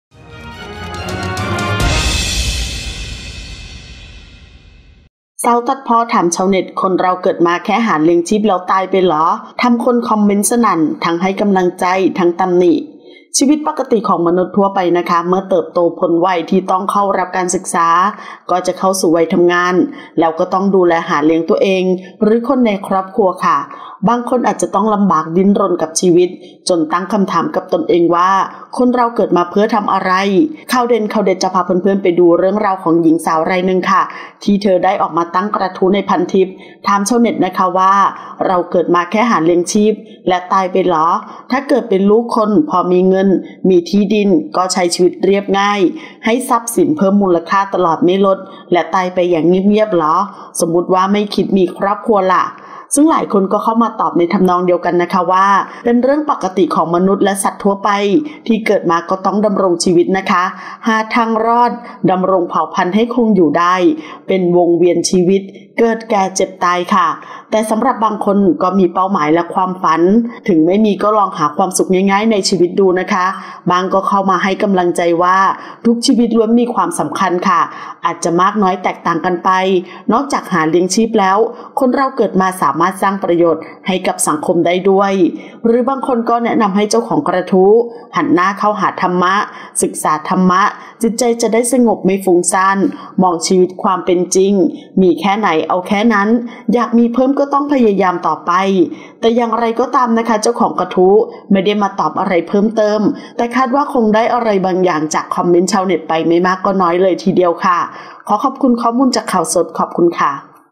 สาวตัดพ้อ ถามชาวเน็ต “คนเราเกิดมาแค่หาเลี้ยงชีพแล้วตายไปเหรอ?” ทำคนคอมเมนต์สนั่น : RECAP
สาวตัดพ้อ ถามชาวเน็ต “คนเราเกิดมาแค่หาเลี้ยงชีพแล้วตายไปเหรอ?” ทำคนคอมเมนต์สนั่น ทั้งให้กำลังใจ ทั้งตำหนิ : RECAP

00:00 - Intro
00:10 - News
01:00 - Minute